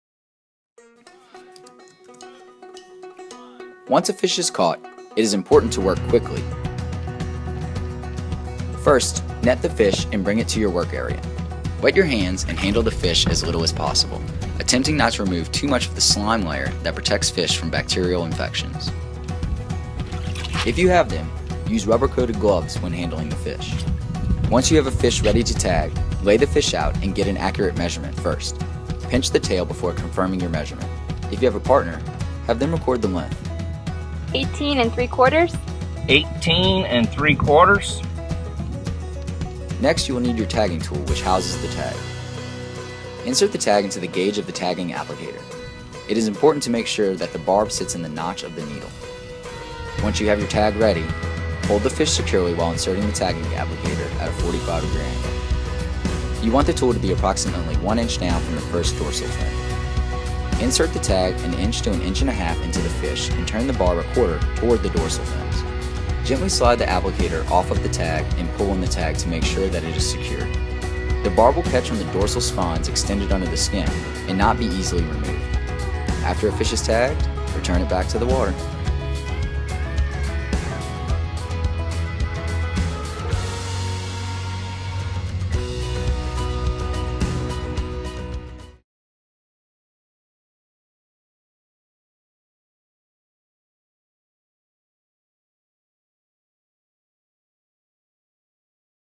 TAG Louisiana - Properly Tag a Fish
This video is about how to properly tag a fish while fishing in the TAG Louisiana Program.